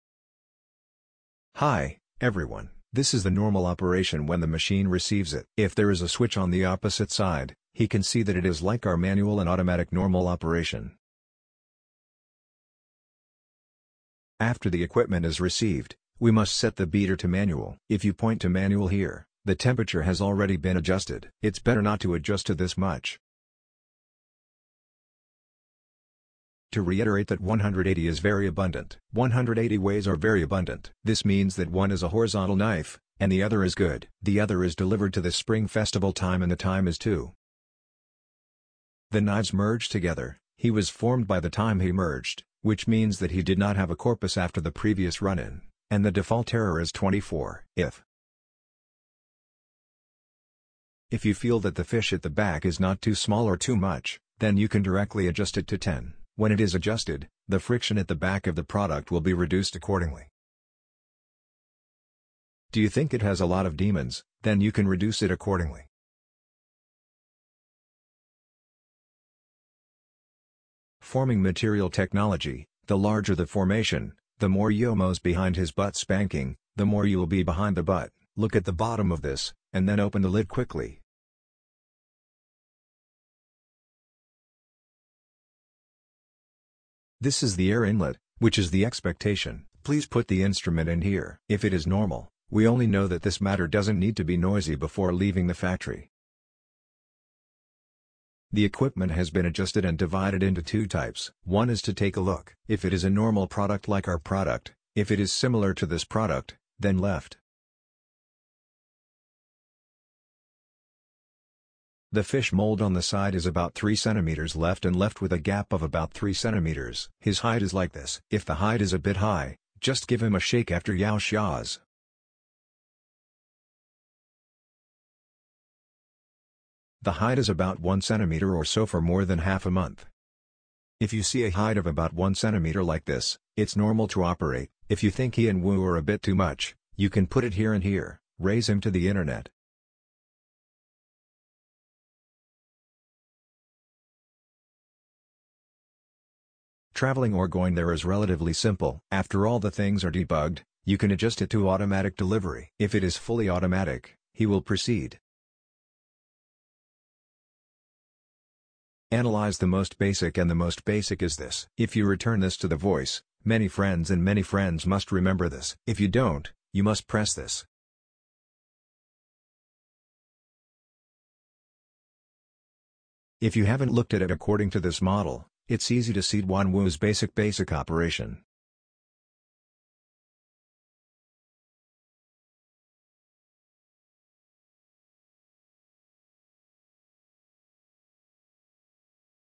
How to operate L bar sealing shrink wrapping machine
Automatic L-shaped sealing and cutting shrinking machine:

Model

FQL-450

FQL-550

Packing Material

PO,POF or PE three layer extruded shrink film roll

Power Supply

380V/50HZ or 220V/60HZ 3PH

380V/50HZ or 220V/60HZ 3PH

Total Power

8.5KW

12.35KW

Sealing Length＆Width

560*320*125mm(L*W*H)

650*420*125mm

Contract the speed

0-10m/min

0-10m/min

Produce the ability

20-30 packs/min

20-30 packs/min

Thickness*width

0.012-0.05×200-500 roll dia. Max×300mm

Air Consumption

0.5MPA

0.5MPA

Overall Dimensions

4000*850*1400mm(L×W×H)

4500*850*1400mm

Weight

550kg

570kg